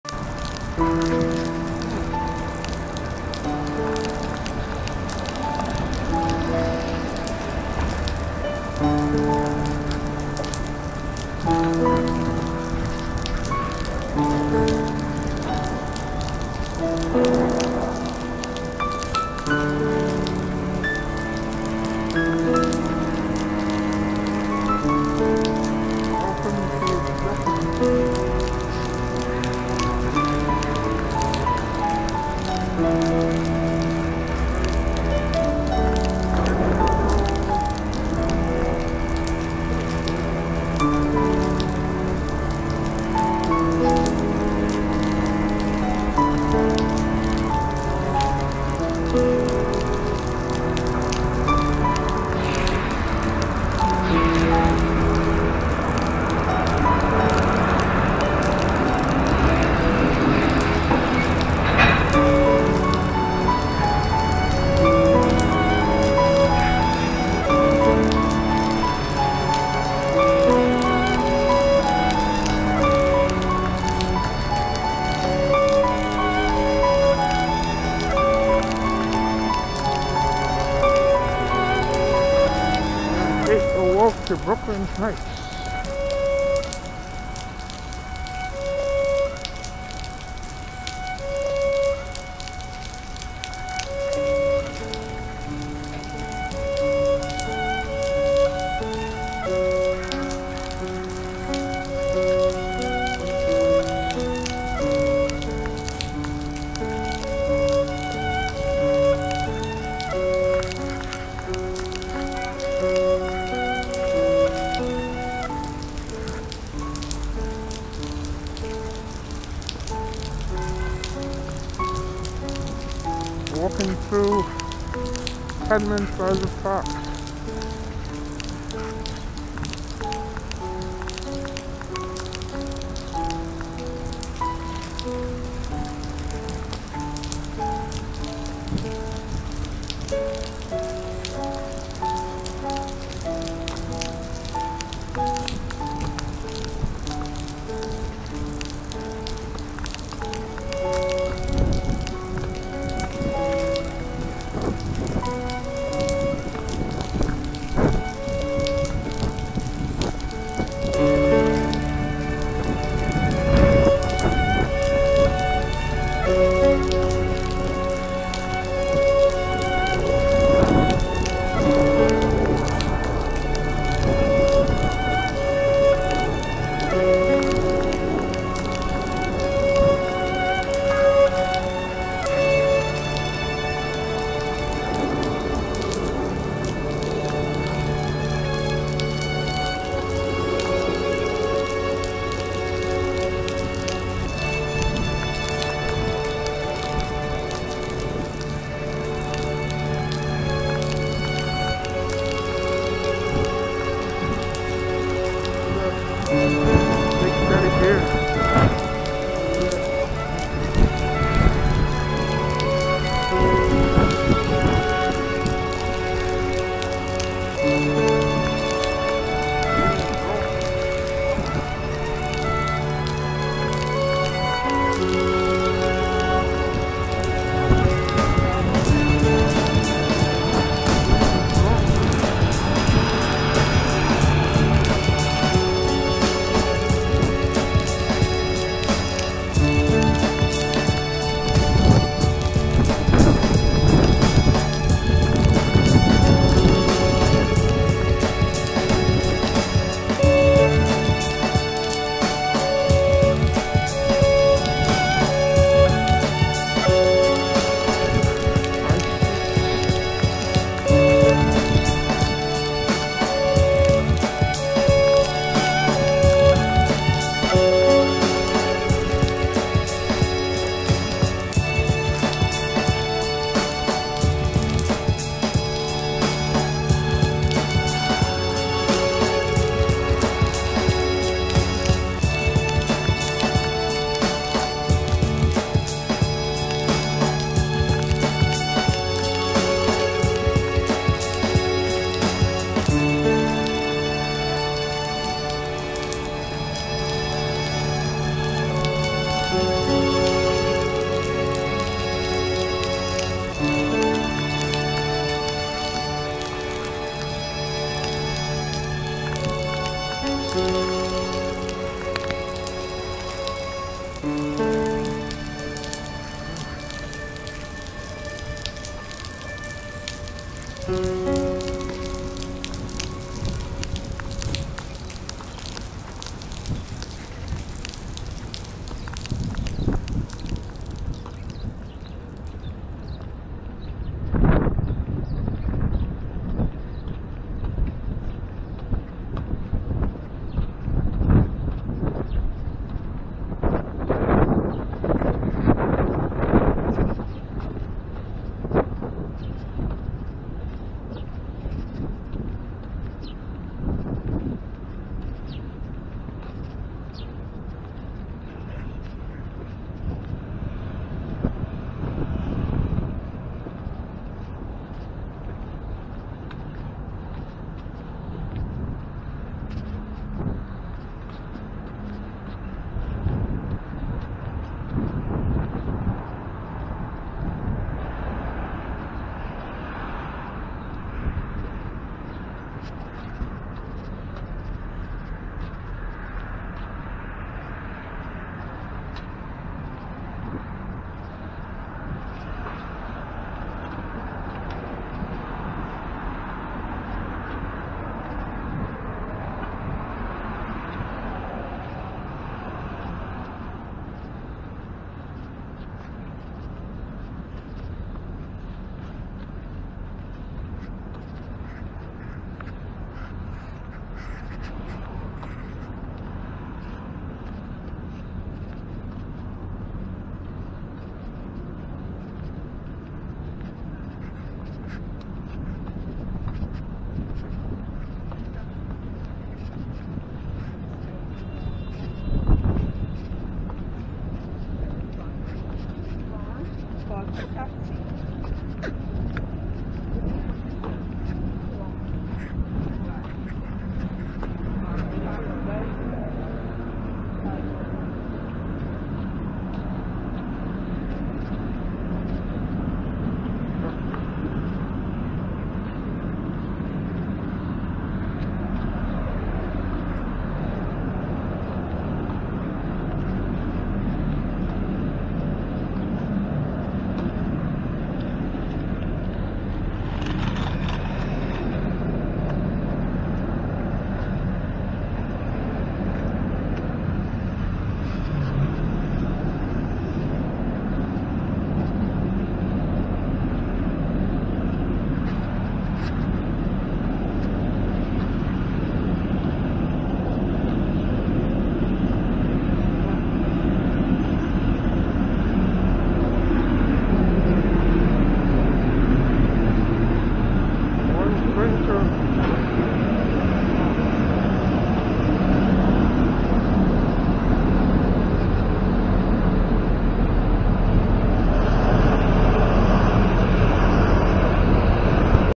Walking through Brooklyn Heights Part 1 of
Walk through Brooklyn Heights starting at Cadman Plaza Park.

Camera: Orgoo Swift